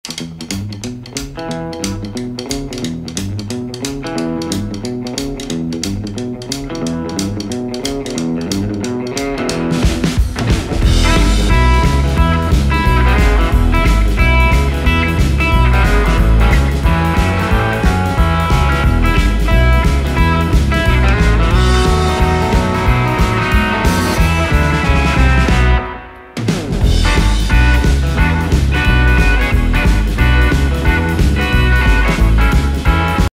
🎵TRUMP DANCE PARTY!!🎵
Watch Trump "do the Donald", which looks a lot like instructions for doing a shoe-shine. But it's better than "do the accordion".